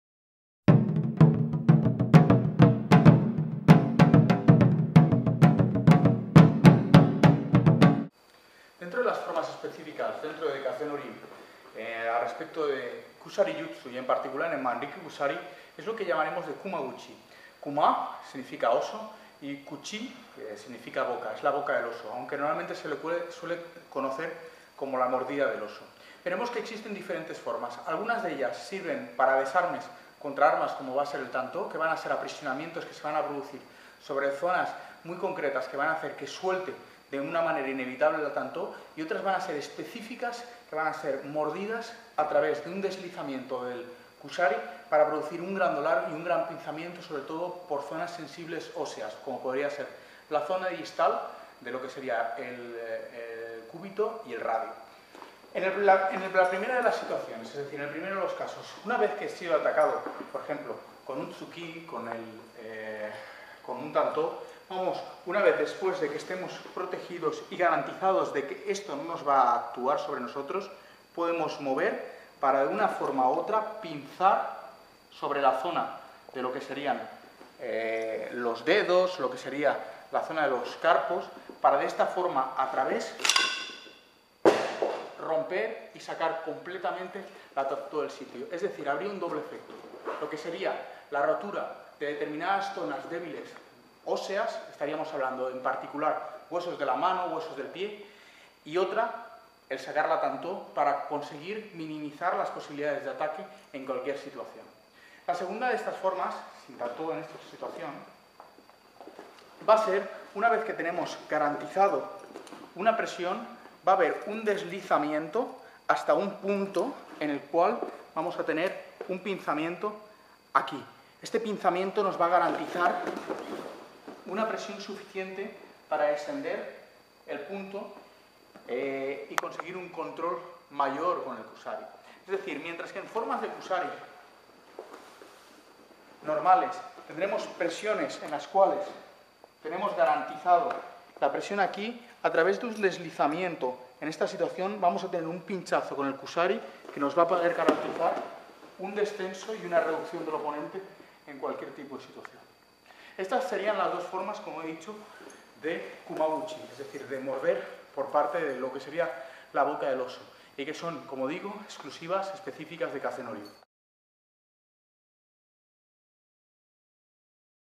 Ogawa Ryu Kaishaku Serie - Manriki Kusari
Luis Nogueira explains Manriki Kusari.